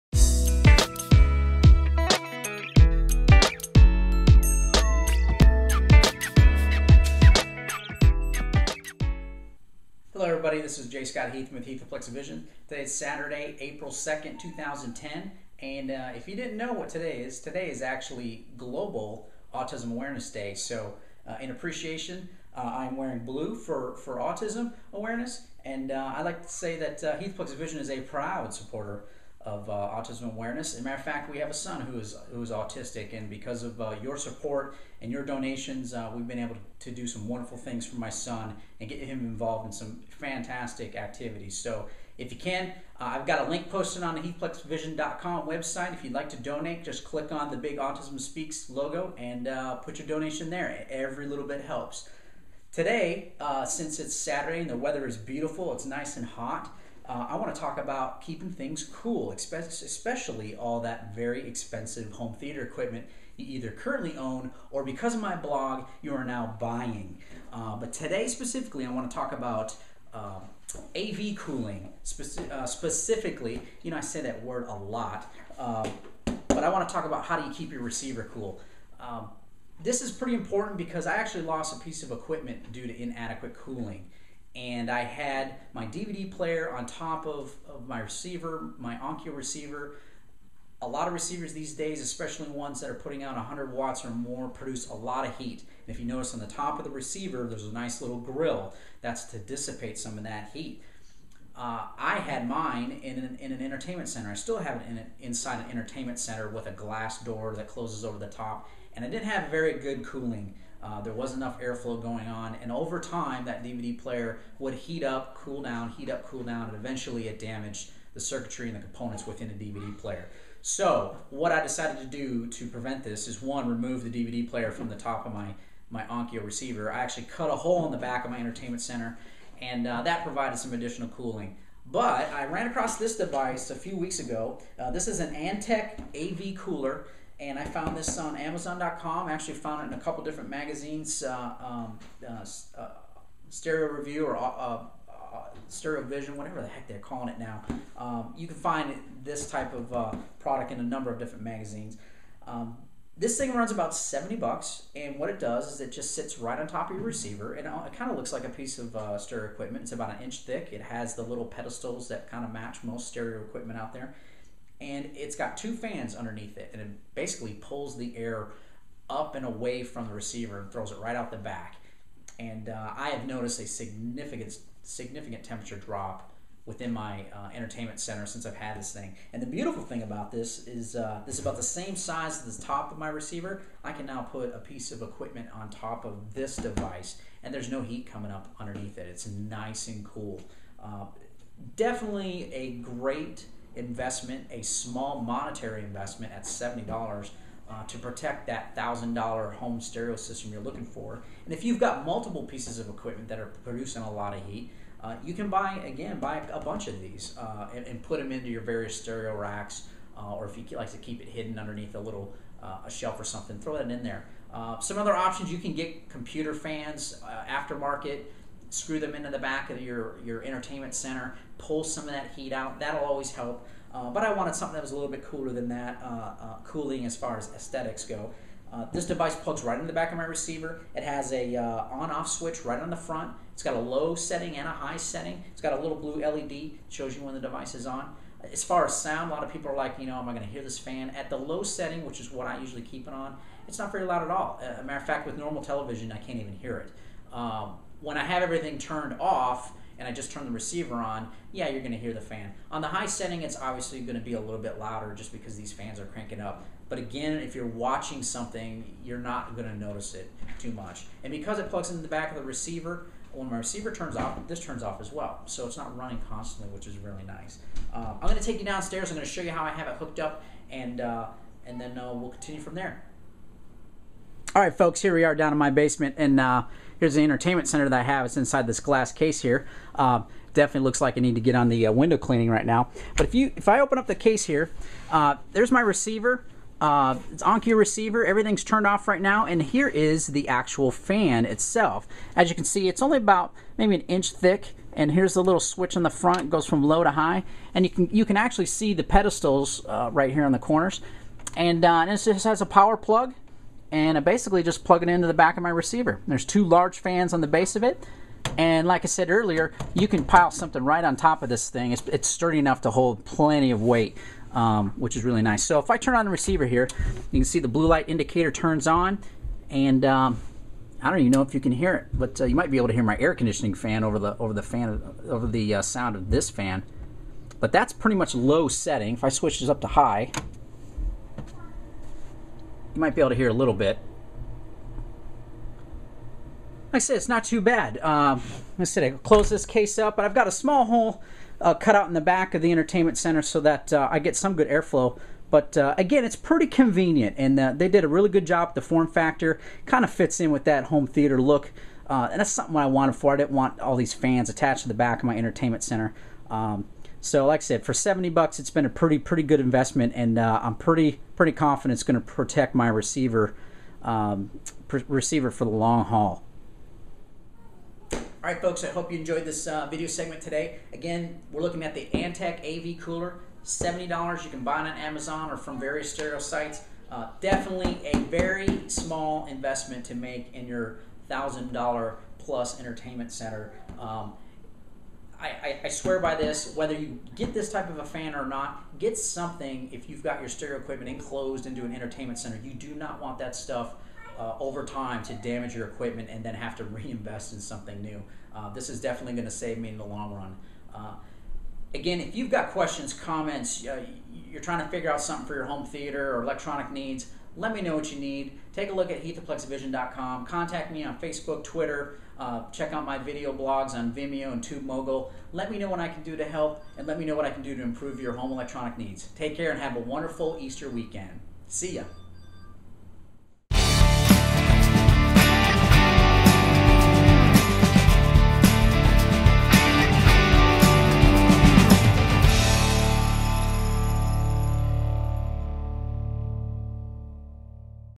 Keep Your Home Theater Cool
heathaplexVISION reviews a fantastic Audio/Video Component Cooler. Antec's Media Component Cooler is the smart choice for keeping you A/V equipment cool. Protect your investment with proper A/V cooling.  Distributed by Tubemogul.